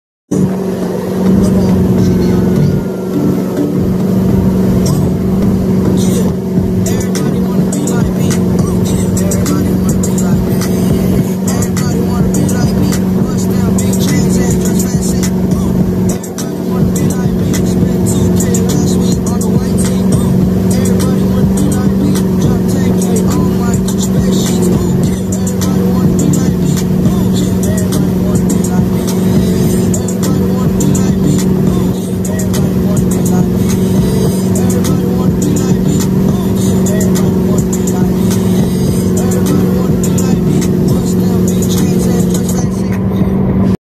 Woman Casually Drinks Coffee And Reads Magazine While Wake surfing HD
This woman added an unconventional spin to wake surfing. Dressed in a bathrobe and with her hair wrapped in a towel, she balanced herself effortlessly on the wakeboard as she sipped coffee from a mug. She then flipped out a magazine and pretended to read it while riding the waves on her surfboard.